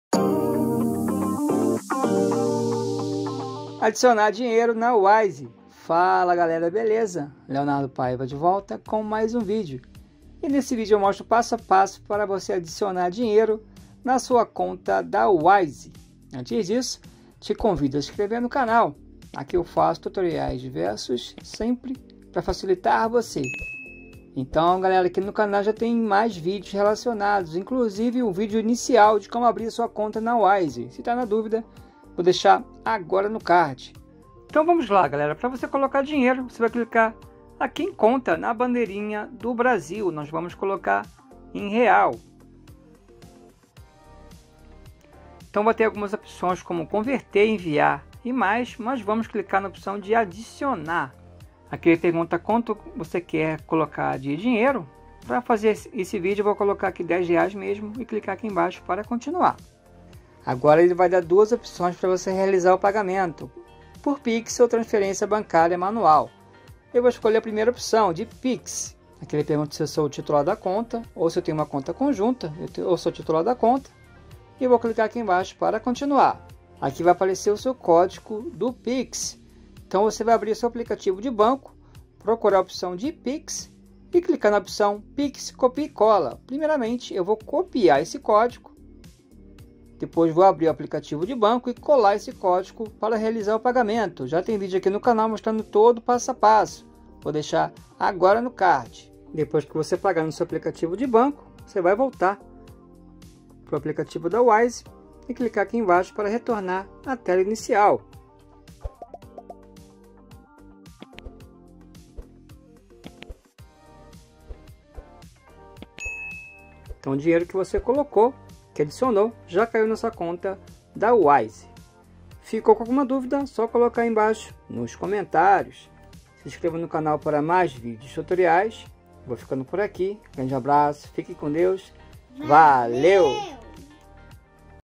ADICIONAR DINHEIRO NA WISE | PASSO A PASSO
Vídeo Tutorial de hoje explico o passo a passo para adicionar dinheiro na sua conta da Wise. A conta da Wise é uma conta multimoeda que permite o envio e recebimento de dinheiro ao redor do mundo. Através dela, é possível guardar e converter mais de 50 moedas diferentes a qualquer momento e em poucos segundos, independente do país.

Neste Vídeo:
Adicionar dinheiro na wise